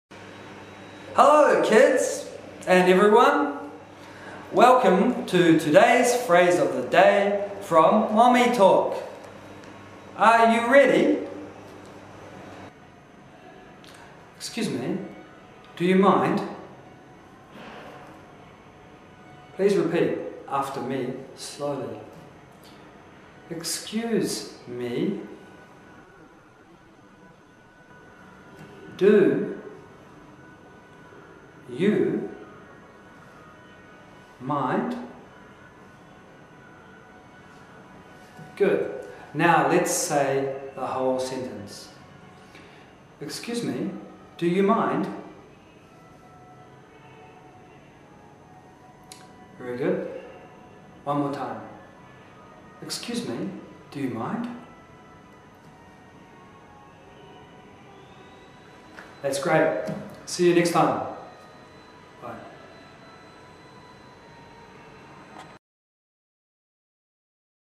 本日のワンフレーズ「すみません。いいですか？」
「すみません。いいですか？」という意味になります。「Do you mind?」で、相手に許可を求めます。「Do you mind if I join you?」で「ご一緒していいですか？」という意味になります。

「本日のワンフレーズ」の詳細は「マミートークブログ」検索で！